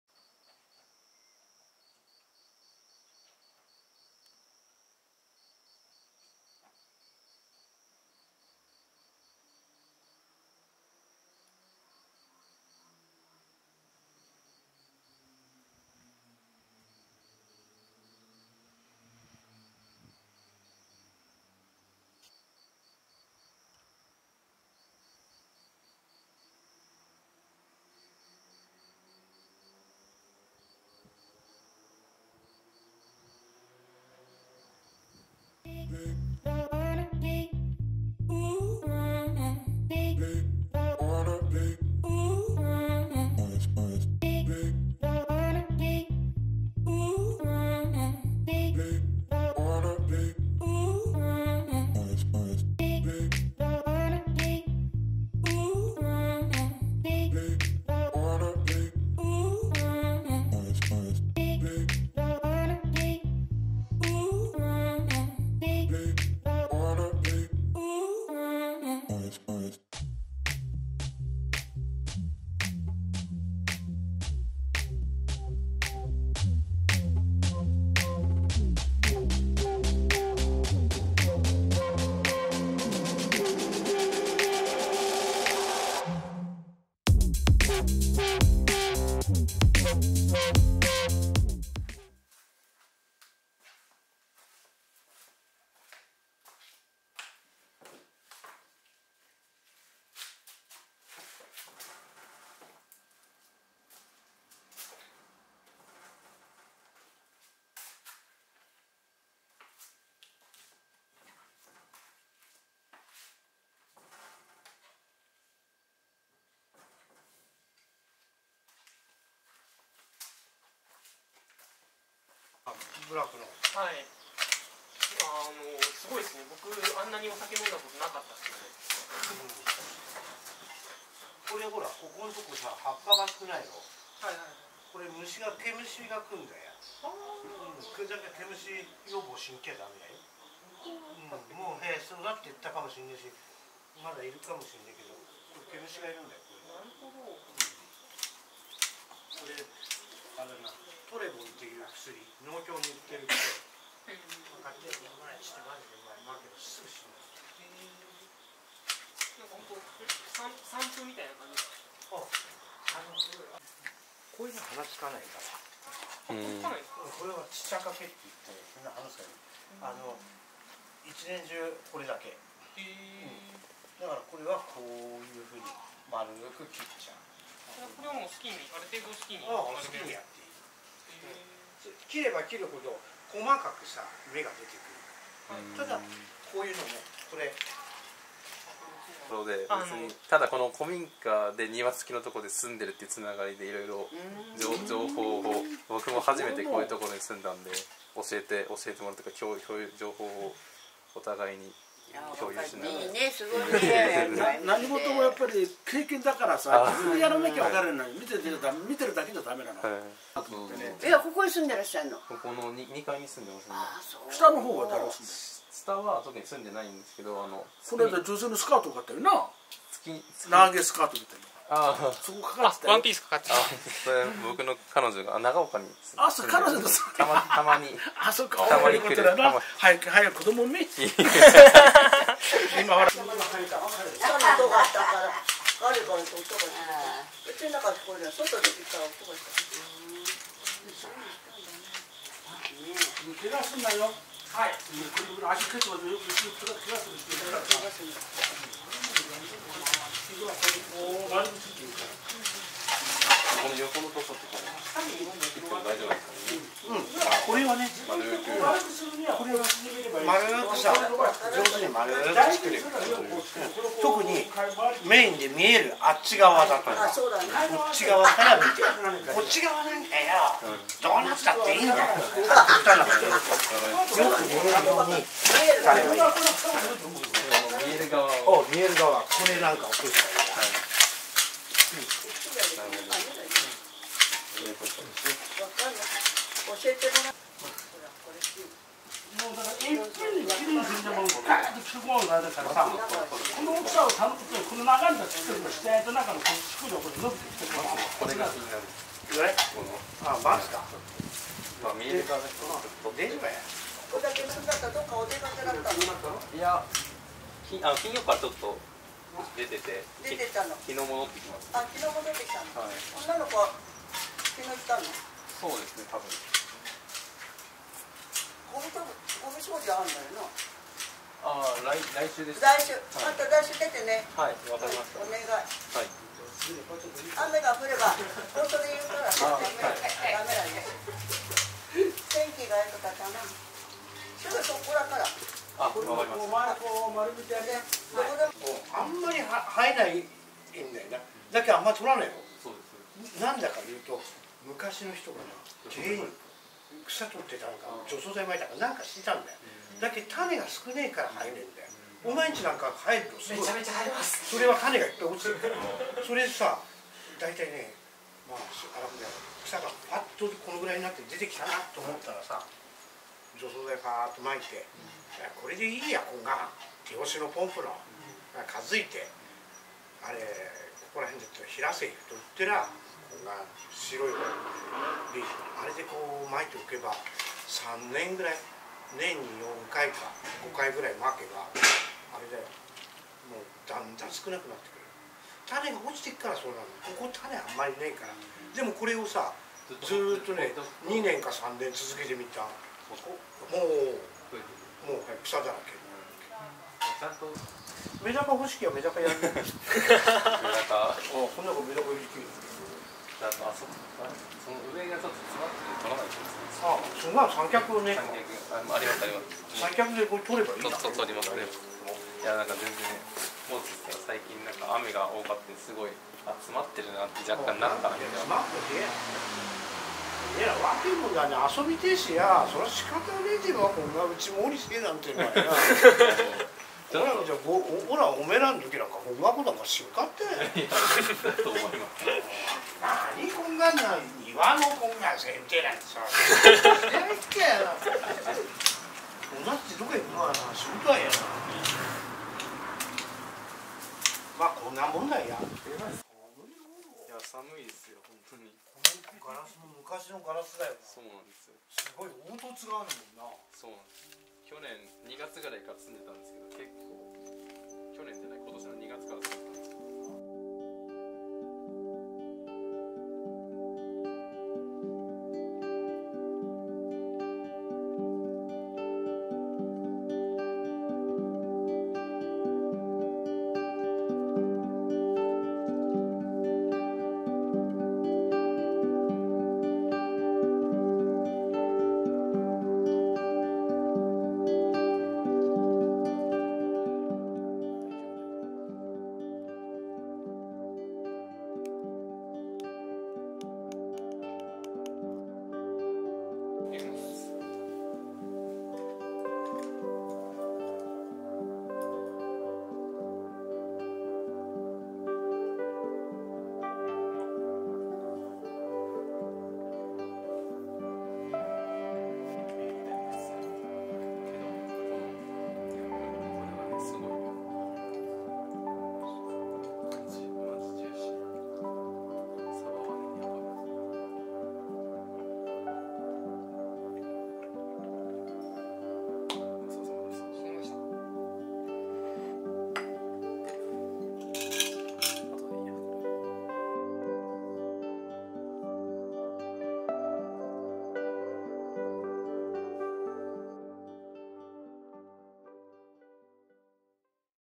【25歳古民家暮らし】25歳素人が築100年古民家の庭づくりをはじめました。
ずっと見てみぬふりをしてきた
新津邸の庭を手入れしはじめました。

偶然の縁で庭師の師匠ができました。
秋になって涼しくなった朝に
庭づくりのレクチャーを受けながら
草取り、剪定を行いました。

庭を管理する上でのノウハウもお伝えしていますが、
地方移住、古民家暮らしのノウハウや
リアルな情報も出てきますので
地方移住や古民家暮らしに興味がある方は必見です。

【使用機材】
・SONY・α７III

・SONY・SEL28F20

・SIGMA 単焦点広角レンズ Art 35mm F1.4 DG HSM

・SIGMA マウントコンバーター MC-11